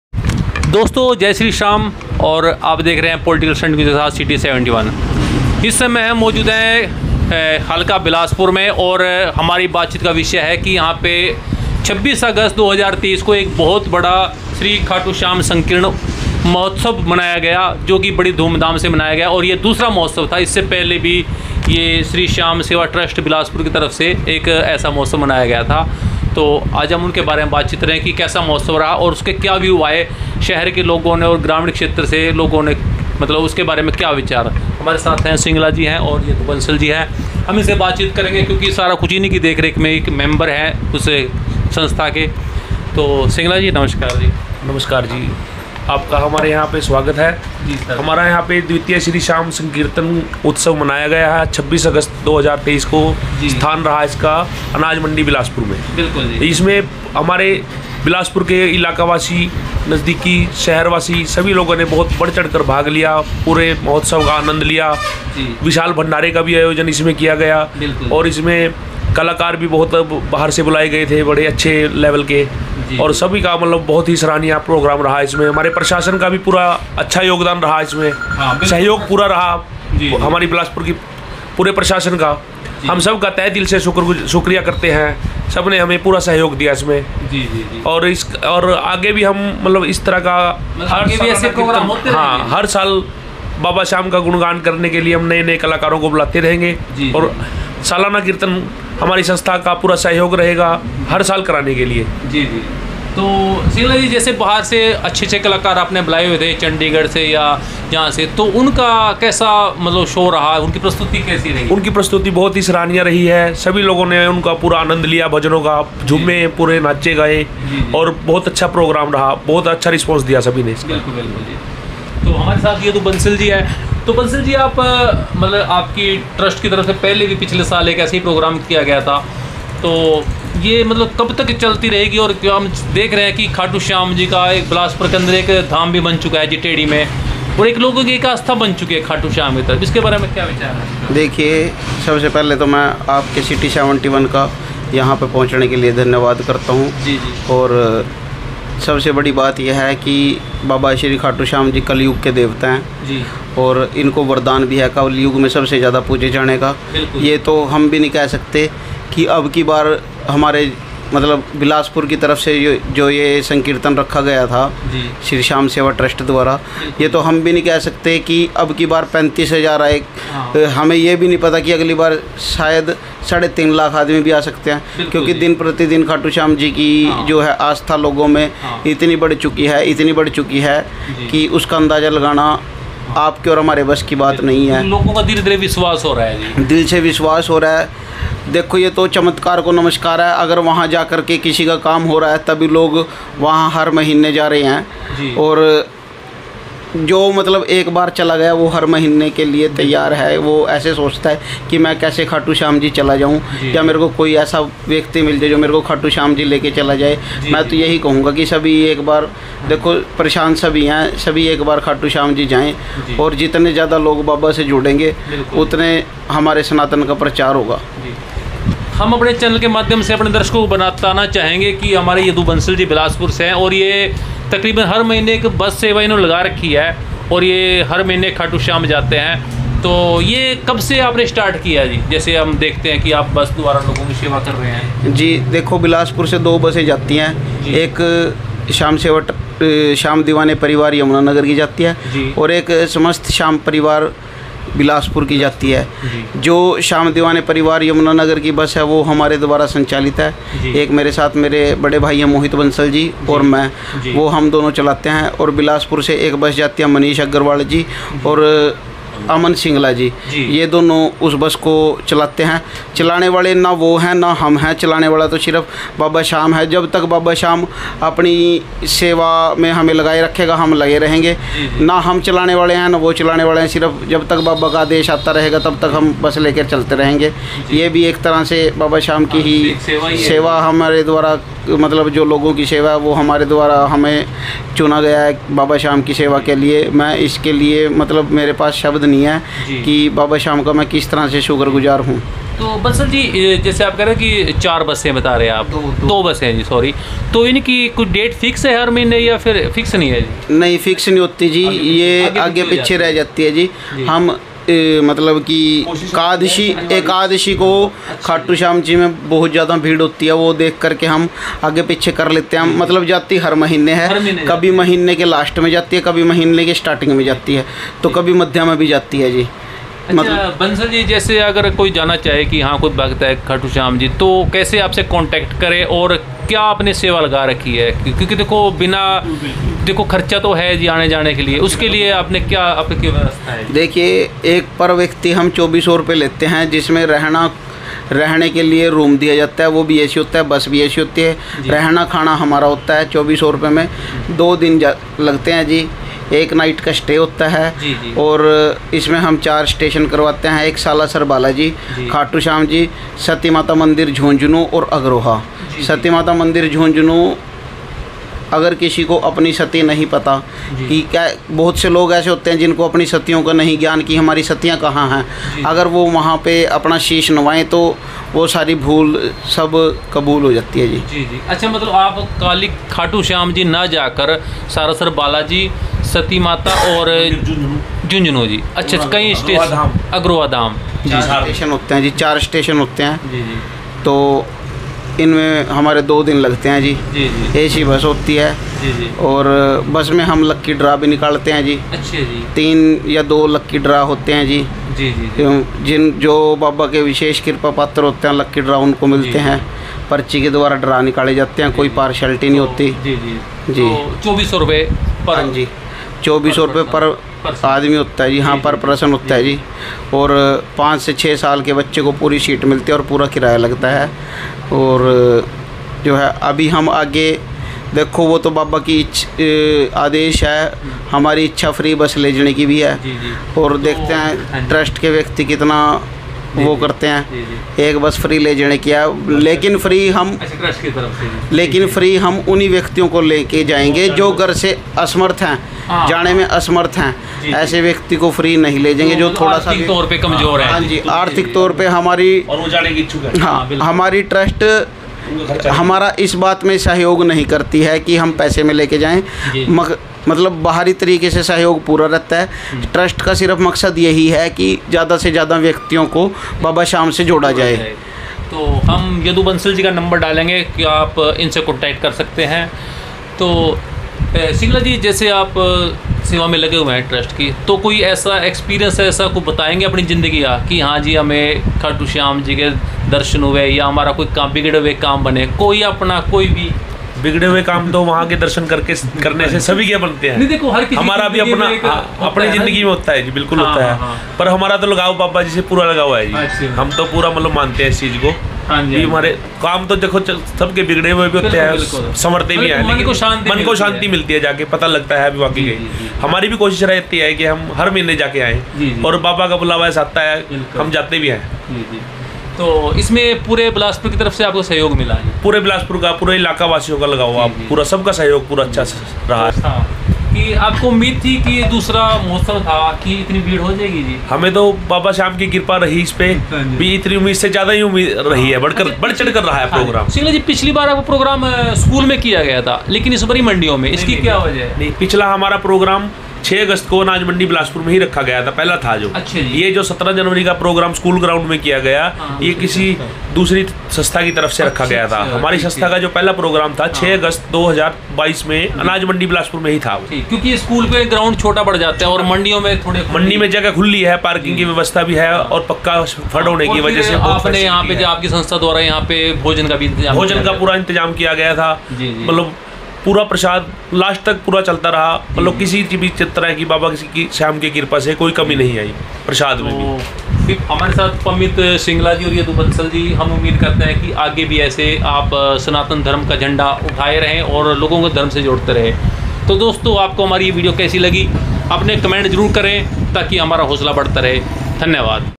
श्री श्याम सेवा ट्रस्ट के कार्यकर्ताओं ने श्री श्याम महोत्सव पर अपने अनुभव शेयर किये।@city71
श्री श्याम महोत्सव पर भक्तों ने भारी संख्या में भाग लिया।